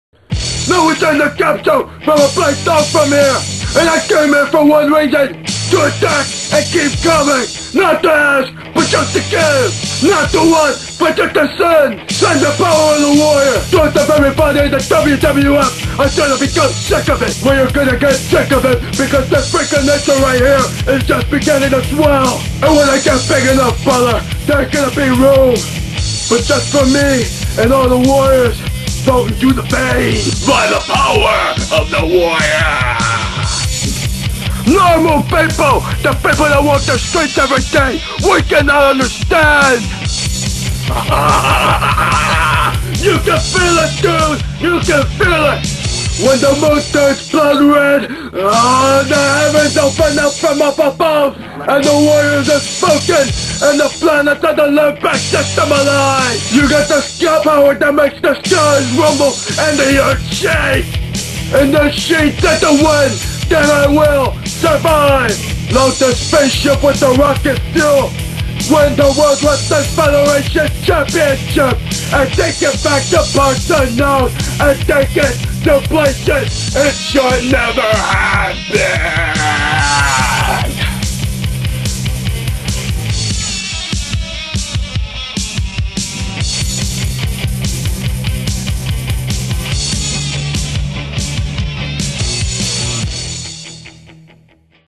The Best Ultimate Warrior Impression EVER !!!
This is me doing an impression of one of my favorite wrestlers from the 1980's THE ULTIMATE WARRIOR